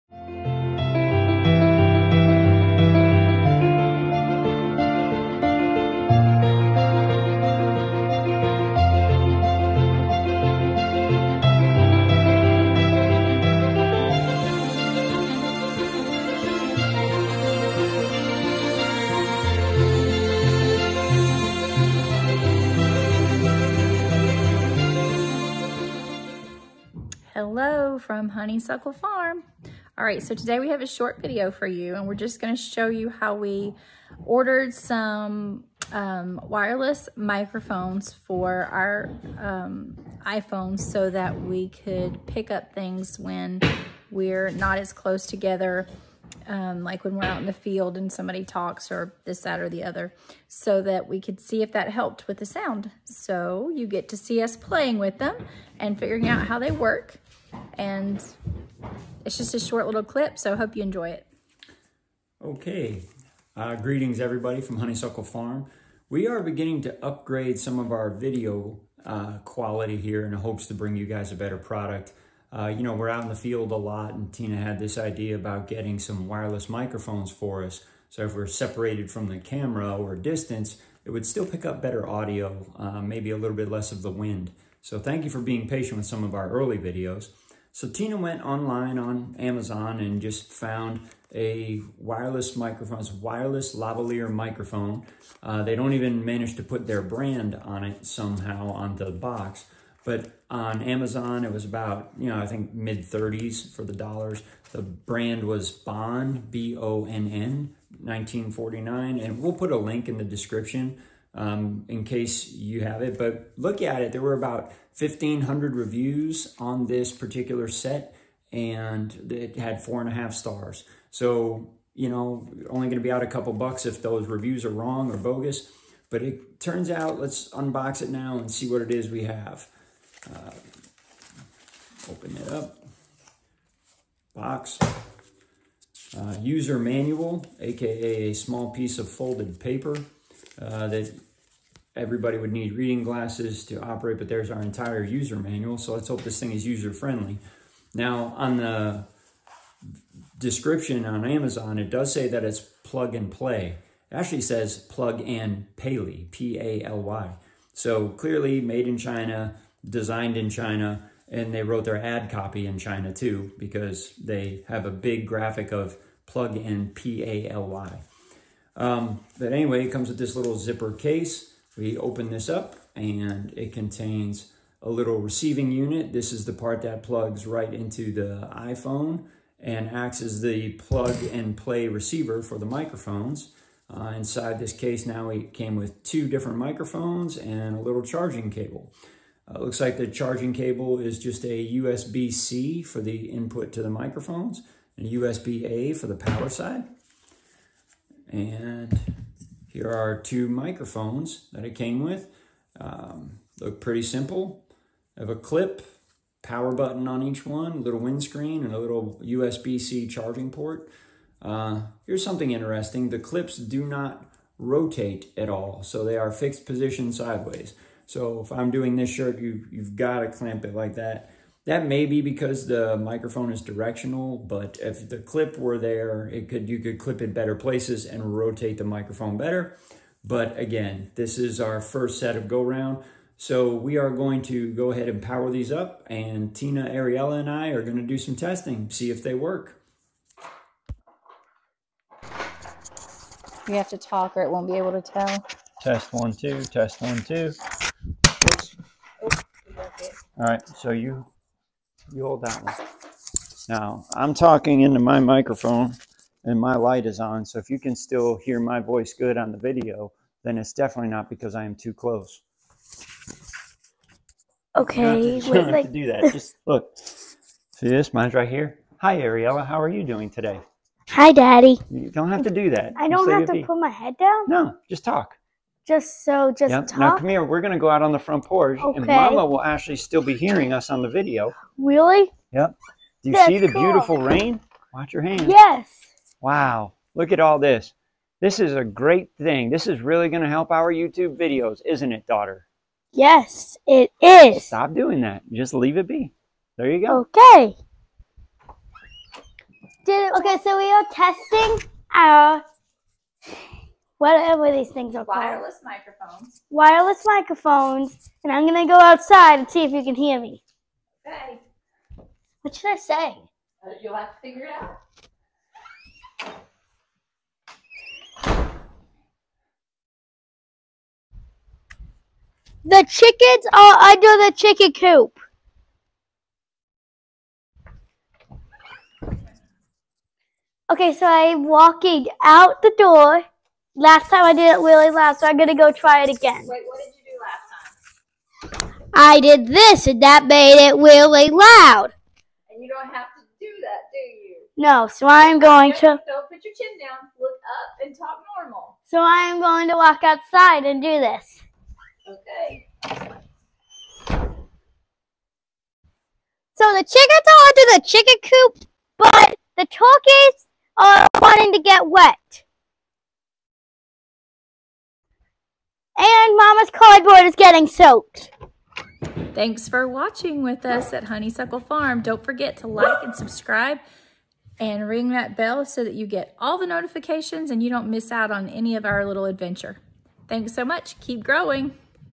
Wireless Mics for the Homestead Unboxing and Trial Run
We got wireless mics so hopefully it helps with the wind, and being away from the phone. we noticed in some of our videos its difficult to hear so we wanted to make it as good as we can! This is the unboxing and a little info on the wireless mics, and the link!
Mya your hands be BUSY and your hearts FULL!
Keep growing!
T